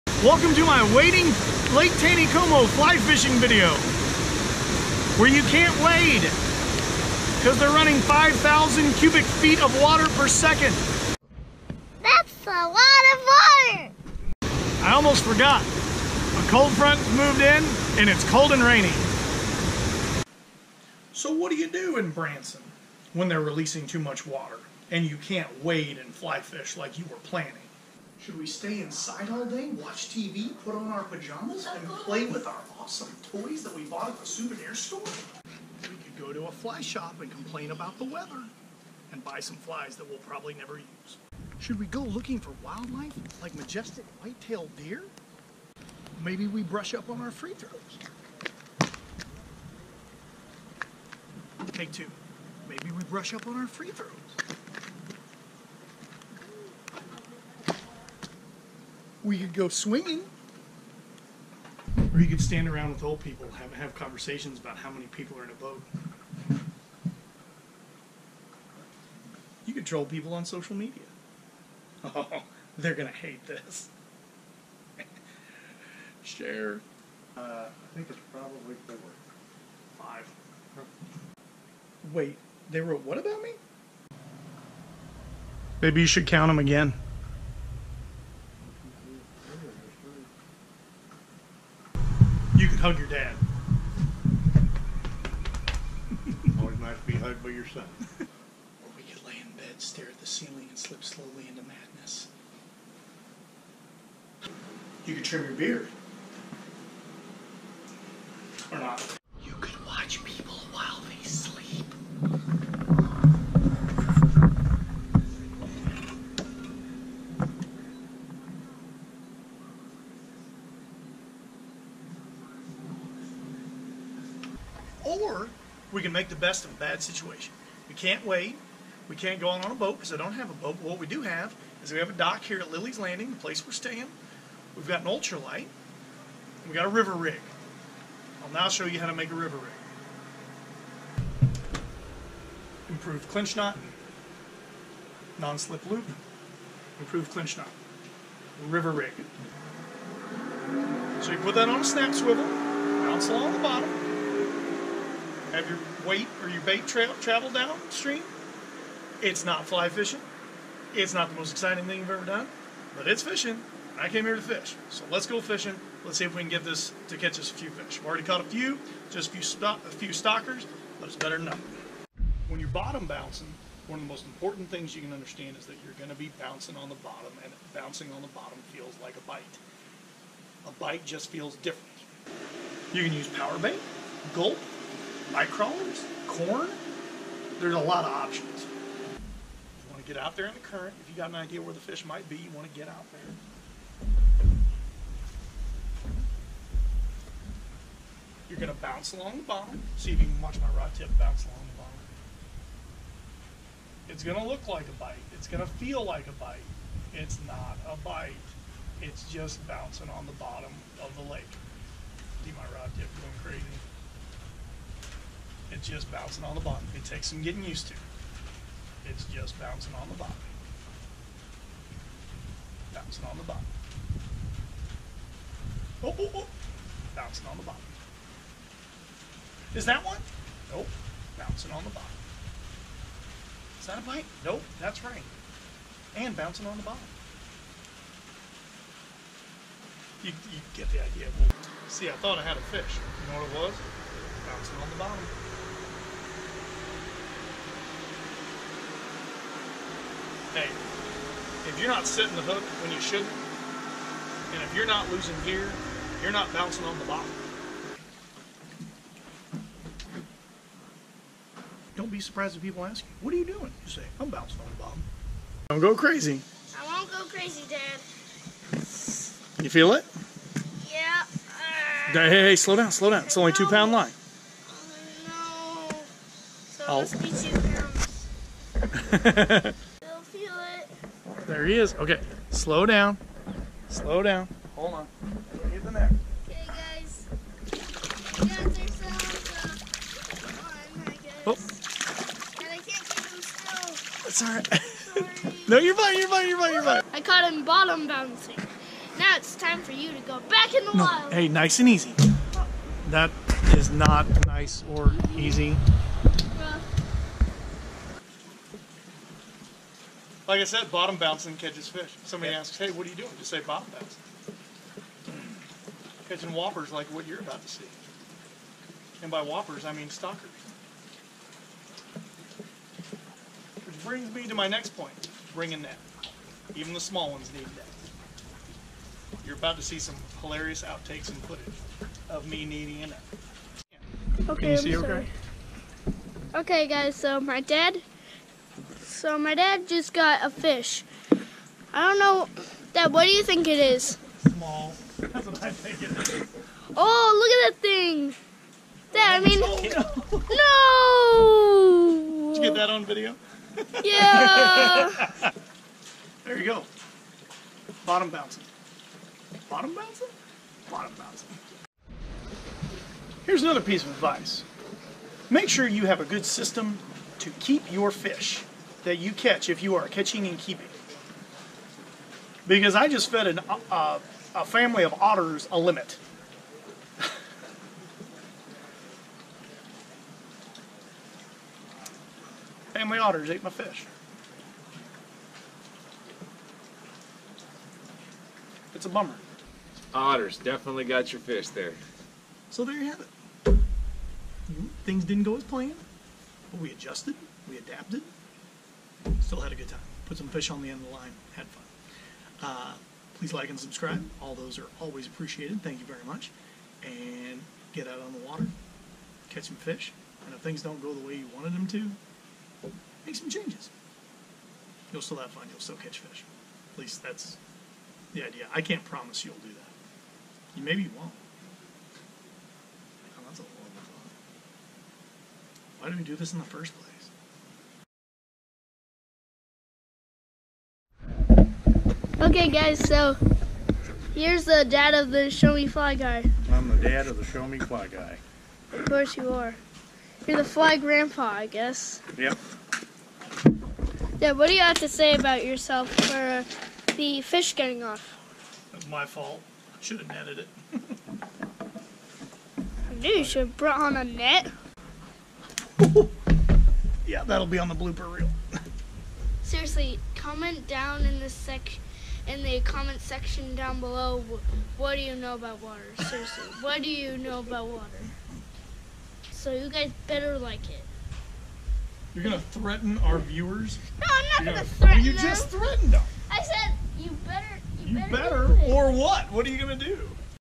Dock Fishing on Lake Taneycomo
Hopefully this video teaches you a little about fishing baits on the bottom of Lake Taneycomo when there is current.  My first trout fishing experience came on Lake Taneycomo.  It took me a long time to learn how to bounce baits off the bottom of the lake and actually catch fish in the process, but hopefully this video makes the process a little easier to understand.